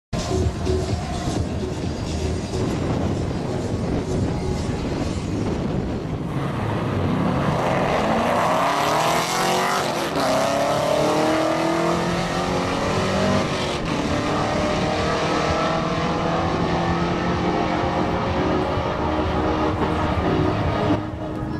2018 Auto Mustang 5.0 Vs. C7 Corvette
90+ Degrees & Bad DA @ Auto Club Dragway in Fontana, CA

2018 10-Speed Automatic Mustang with Tune & 305/35/19 NT05.

C7 Corvette with exhaust & flex fuel dyno tune